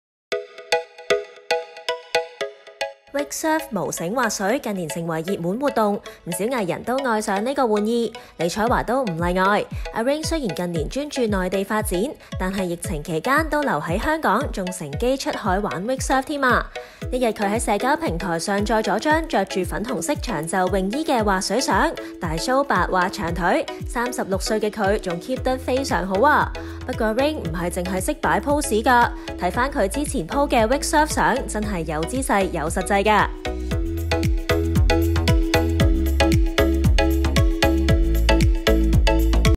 【娛樂】李彩華玩WakeSurf騷長腿
近年專注內地發展的李彩華(Rain)，抗疫期間多留在港，更愛上近年流行的水上活動wakesurf，在其社交平台看到，現年36歲的她還保養得宜，玩起wakesurf還有板有眼。

bmgproductionmusic